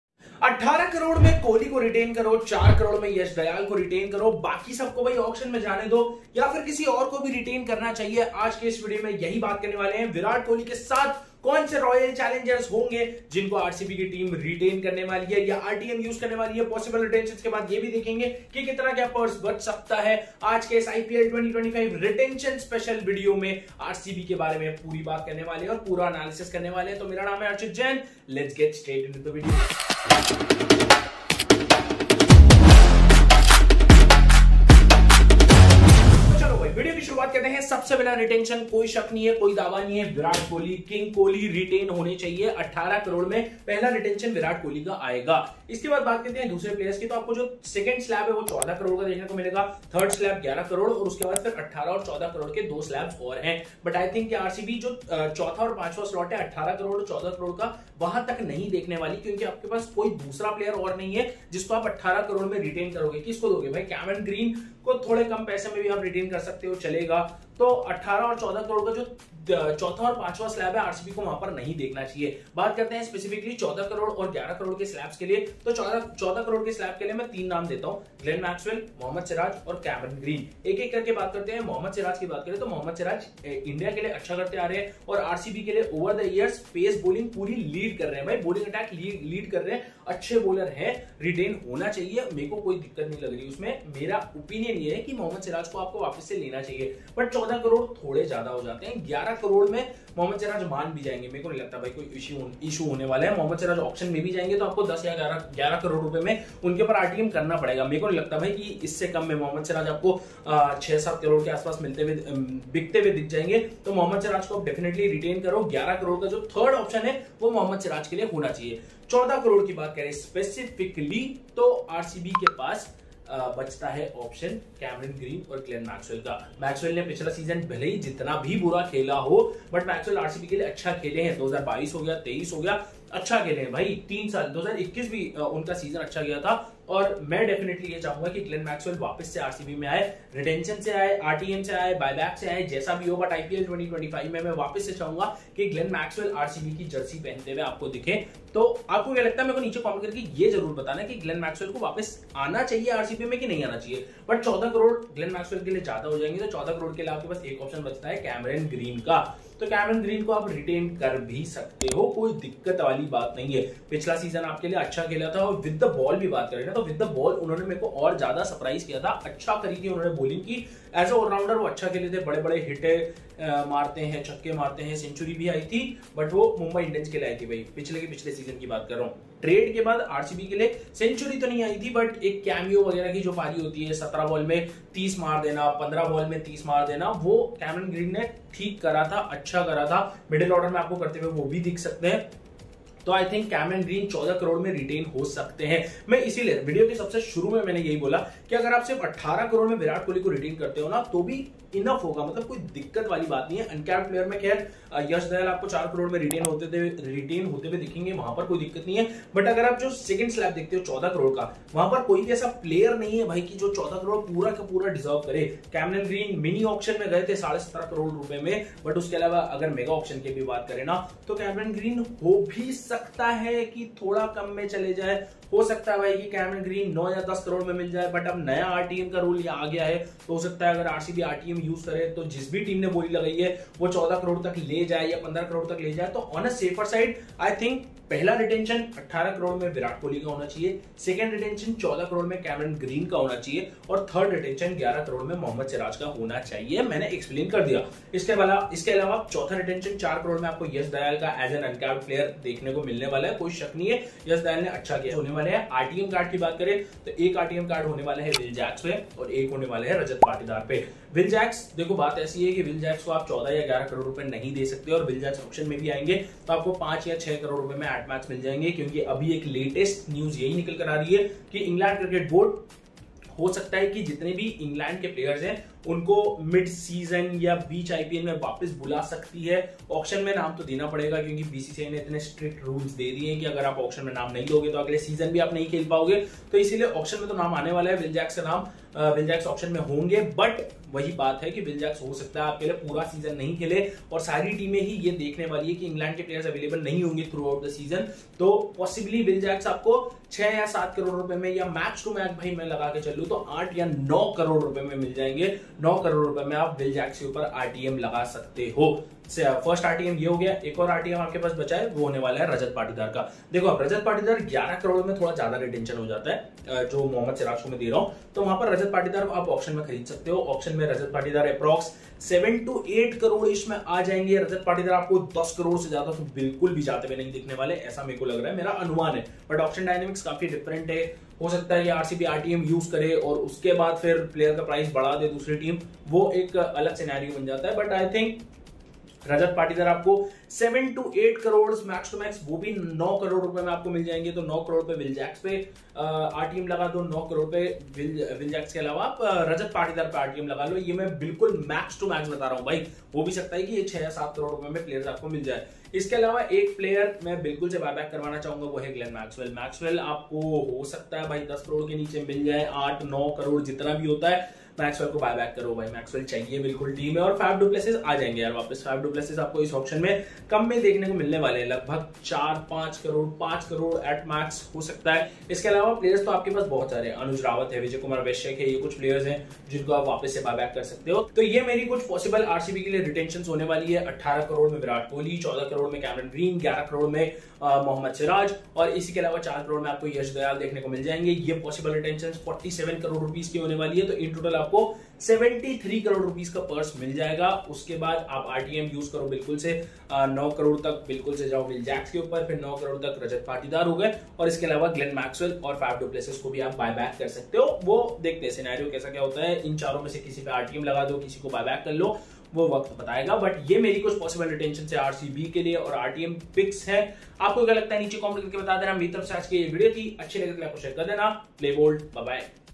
IPL 2025 - ROYAL CHALLENGERS BANGALORE RETAIN PLAYERS LIST || RCB RETENTIONS
In this video, I discuss RCB's retention strategy for IPL 2025! Who will the team retain, and which players are likely to be picked using the RTM card? From top performers to key young talents, I share my opinion on RCB's best options to build a winning squad for next season. Don't miss out on this in-depth analysis of RCB's strategy for the upcoming IPL auction!